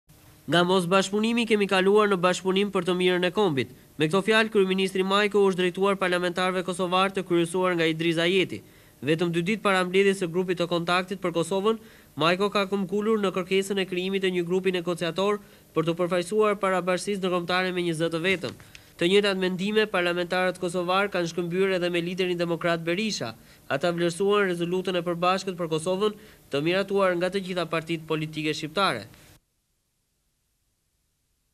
Majko takim me parlamentarët kosovarë (26 Janar 1999)
Arkiva - Tv Klan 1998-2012; Pamje nga takimi i Kryeministrit Pandeli Majko me parlamentaret kosovare te kryesuar nga Idriz Ajeti.Pamje nga takimi i Kryetarit te Partise Demokratike Sali Berisha me parlamentaret kosovare te kryesuar nga Idriz Ajeti.

Nga mosbashkëpunimi kemi kaluar në bashkëpunim për të mirën e kombit." Me këto fjalë Kryeministri Majko u është drejtuar parlamentarëve kosovarë të kryesuar nga Idriz Ajeti. Majko ka këmbëngulur në krijimin e një grupi negociator për t'u përfaqësuar para bashkësisë ndërkombëtare me një zë të vetëm. Të njëjtat mendime parlamentarët kosovarë kanë shkëmbyer edhe me liderin demokrat Berisha. Ata vlerësuan rezolutën e përbashkët për Kosovën të miratuar nga të gjitha partitë politike shqiptare.